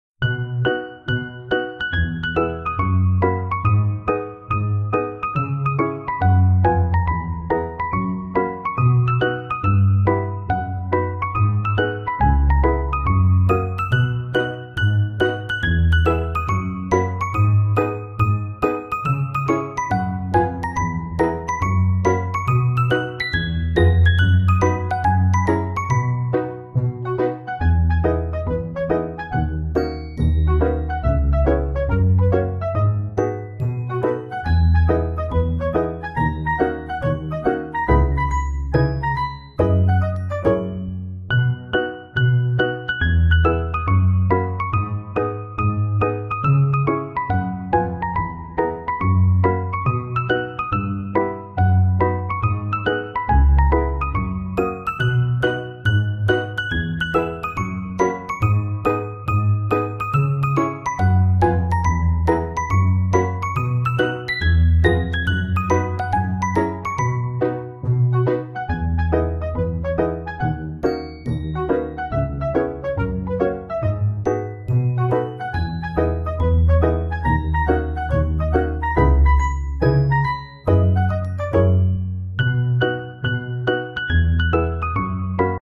POP MART Image of Reality “The Duality” DIY Decoden Phone Case#decoden
CHEROR will teach you how to become a decoden master step by step from a novice in decoden making!